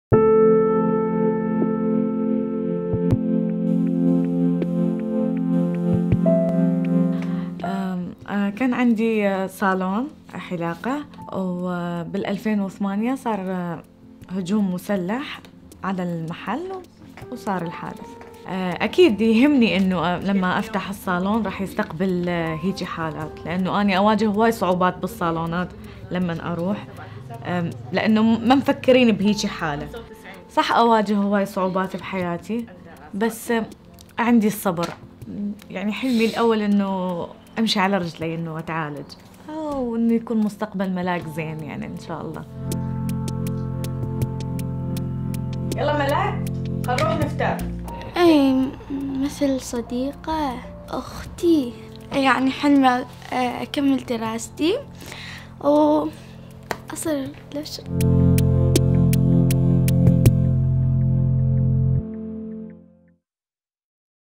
Η ανεξαρτησία και η αυτονομία μου εξαρτώνται από εμένα
Η Zainab έφτασε στη Θεσσαλονίκη από το Ιράκ μαζί με την αδερφή της, Sumar, και τη 12χρονη κόρη της, τη Mallak. Τα τελευταία δέκα χρόνια, χρησιμοποιεί αναπηρικό αμαξίδιο λόγω των σοβαρών τραυματισμών που προκλήθηκαν στη σπονδυλική της στήλη κατά την ένοπλη επίθεση που δέχτηκε το κομμωτήριό της. Στο ταξίδι της από την πατρίδα της, η Zainab αντιμετώπισε αρκετές προκλήσεις, αλλά η οικογένειά της τη βοήθησε προκειμένου τελικά να βρει ασφάλεια.

Από τότε που έφτασε στην Ελλάδα, η κόρη της, η Mallak, πηγαίνει σχολείο και είναι εξαιρετική μαθήτρια. Ονειρεύεται όταν μεγαλώσει να γίνει διερμηνέας, καθώς έχει πάθος για τις ξένες γλώσσες, ή αστυνομικός, ώστε να μπορεί να βοηθάει τους ανθρώπους. Η Zainab λαμβάνει υποστήριξη από το πρόγραμμα REACT του Δήμου Θεσσαλονίκης, εταίρου της  Ύπατης Αρμοστείας,  όπως και τις οργανώσεις ARSIS και Praksis. Όπως δηλώνει, όμως, οι δυνατότητες ένταξης είναι περιορισμένες και οι ανάγκες για εξειδικευμένη ιατρική υποστήριξη και αποκατάσταση μεγάλες. «Αυτό δεν με αποτρέπει από το να ονειρεύομαι να ανοίξω ξανά το δικό μου κομμωτήριο», δηλώνει. «Η ανεξαρτησία και η αυτονομία μου εξαρτώνται από μένα.»